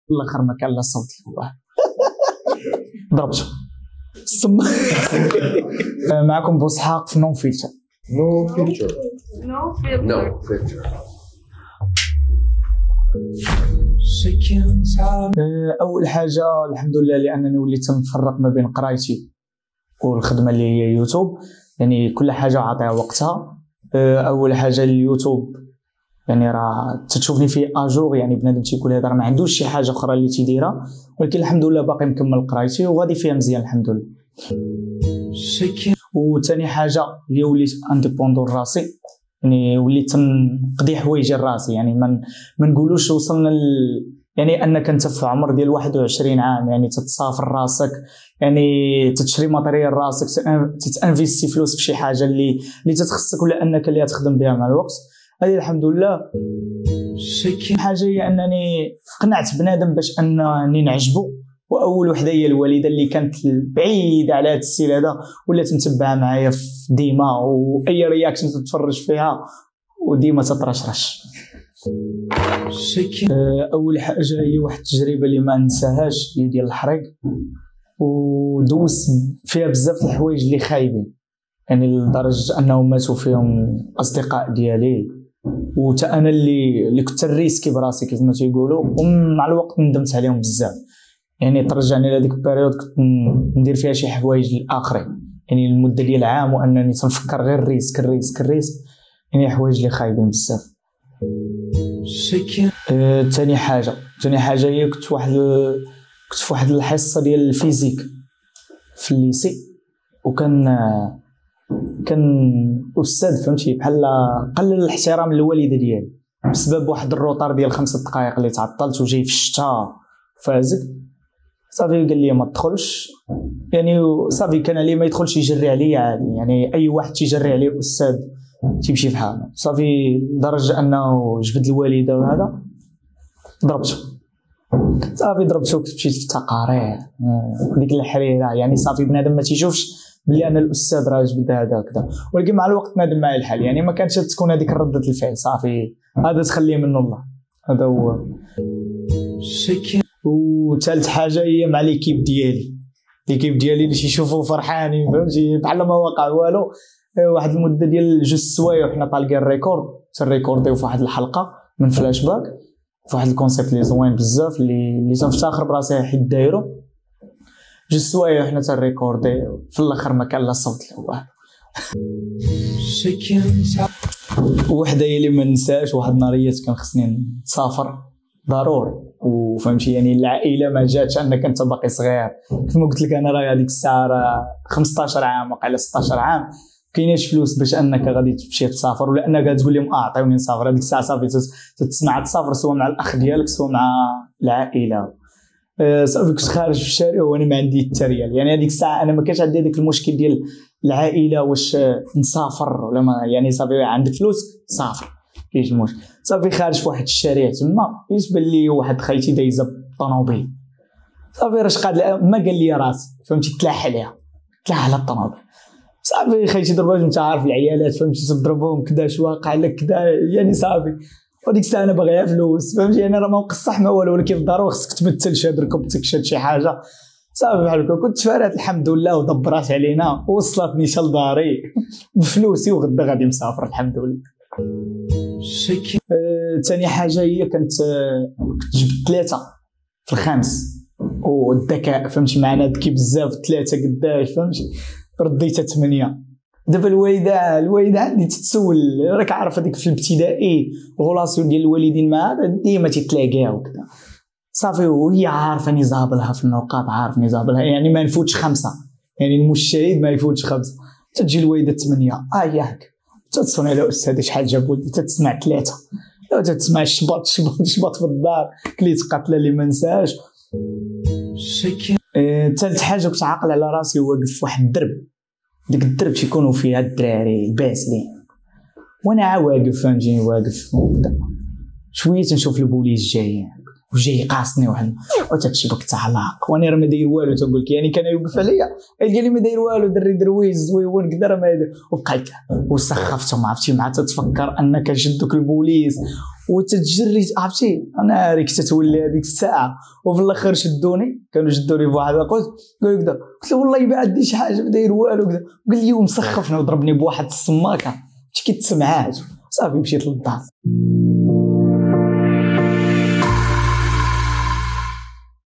No Filter - Youssef Boushaq - كايعاود لينا القصة ديالو مع الحريك ،الشغب و مغامرات الصغر
Dans cet épisode, Youssef Boushaq nous raconte ses 3 Réussites, Échecs et Bêtises.
👇👇 Suivez-nous 👇👇
Site Web Officiel